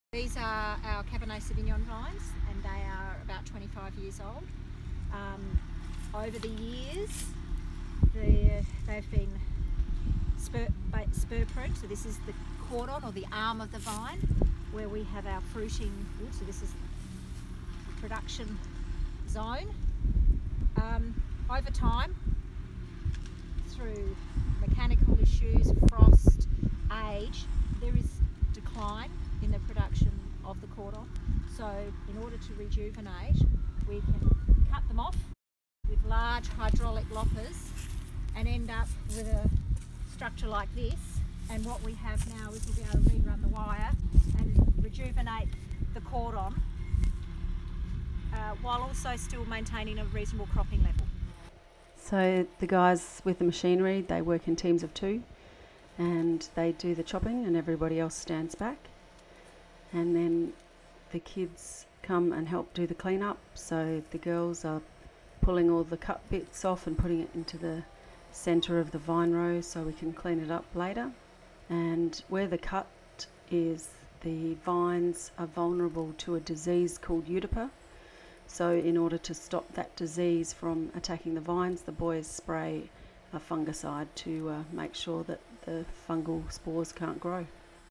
Restructure of Cabernet Vines in winter
Restructuring of Cabernet Sauvignon vines by cutting the arms of the vine off at the cordon and arching 2 new canes on each side to rejuvenate and spread the fruit zone at Eight at the Gate Wines in Wrattonbully South Australia